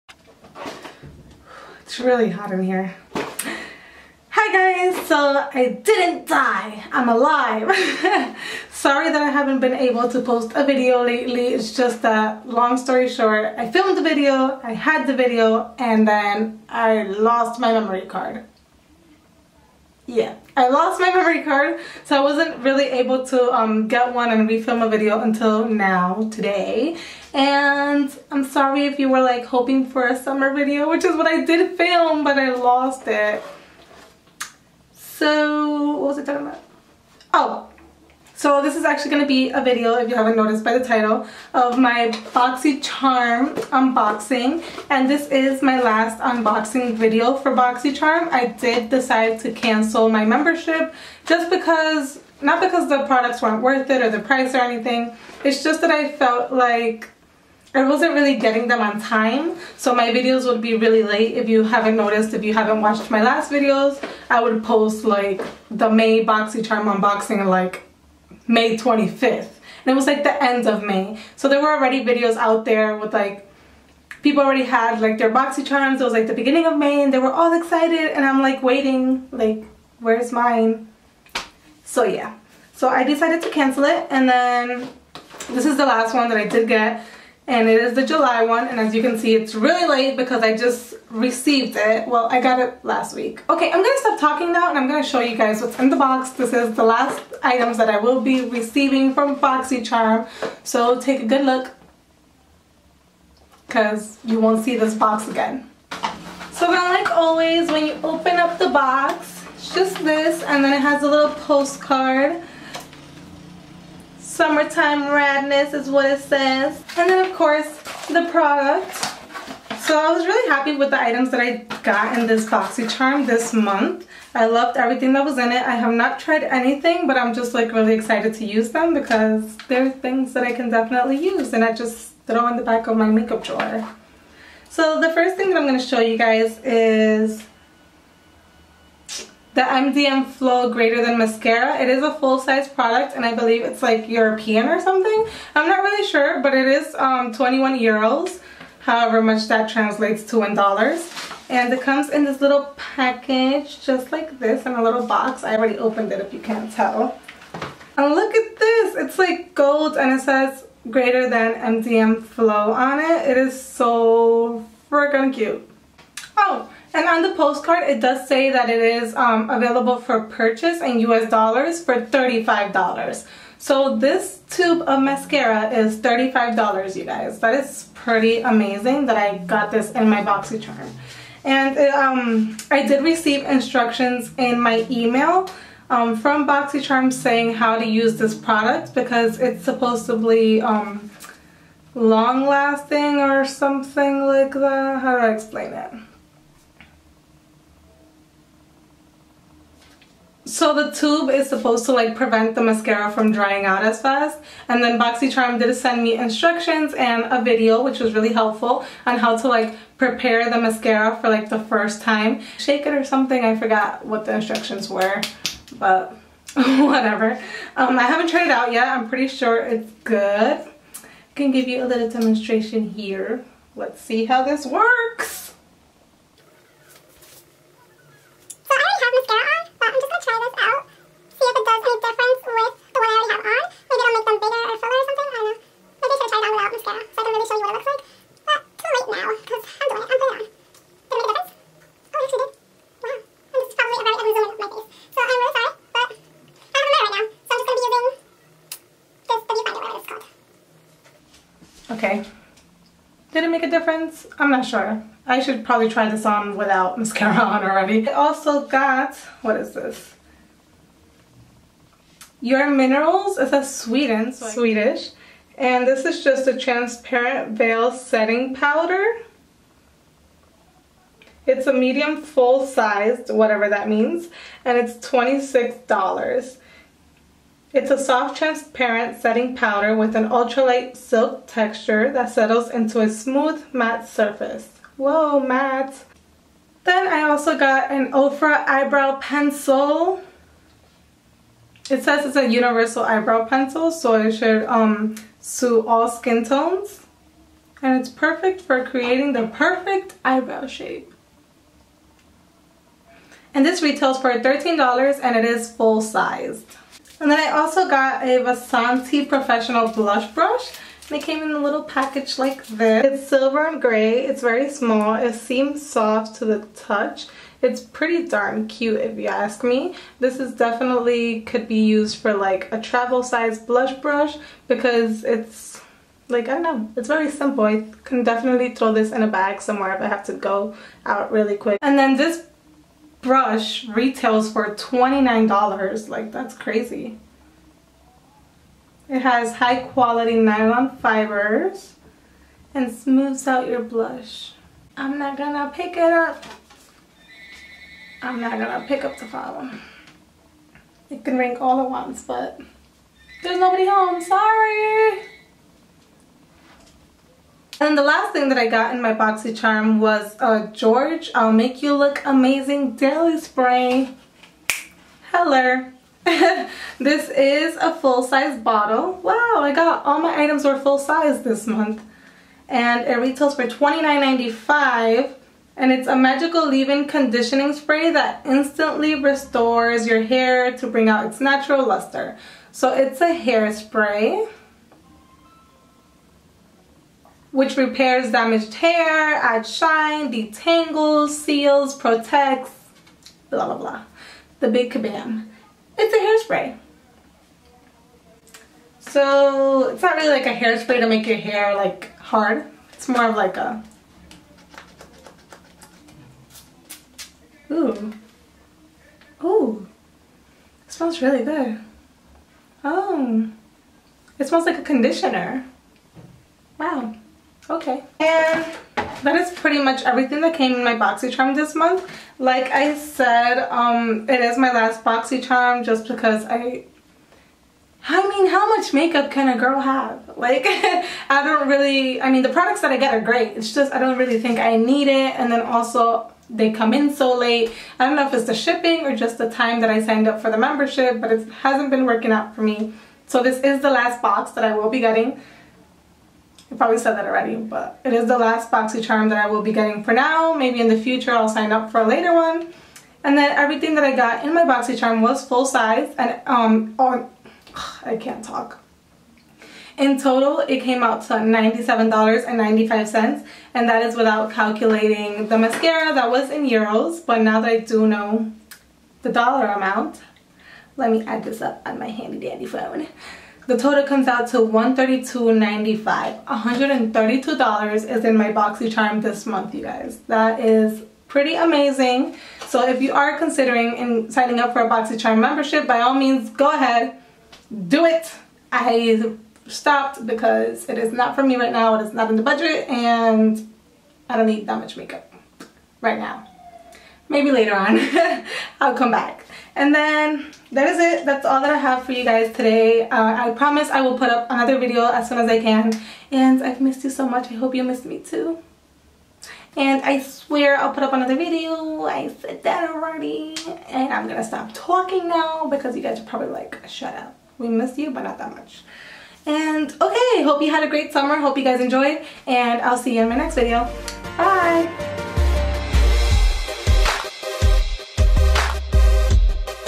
Last Boxycharm Unboxing | July
This video is the last Boxycharm unboxing that I will be doing.. until further notice. I do explain why, and it is not because I am dissatisfied with the products or subscription in anyway.